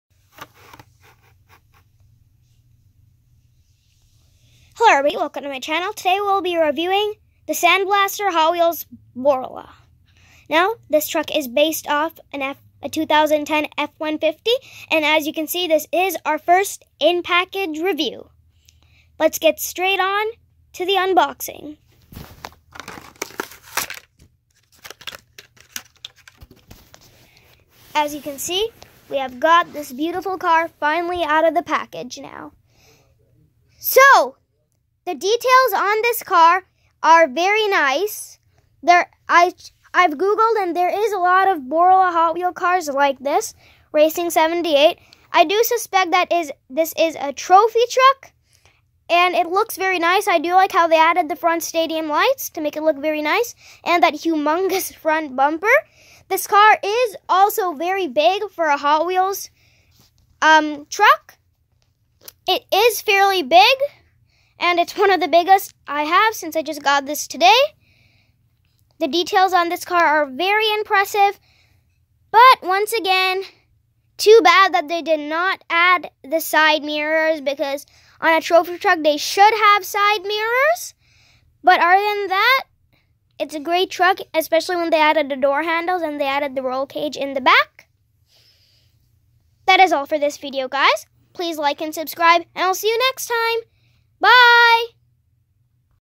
Borla Truck Review
Hi everyone , On this video I'll be reviewing this Awsome Truck. This is one of the biggest truck I own in my collection. WHAT A BEAST 😎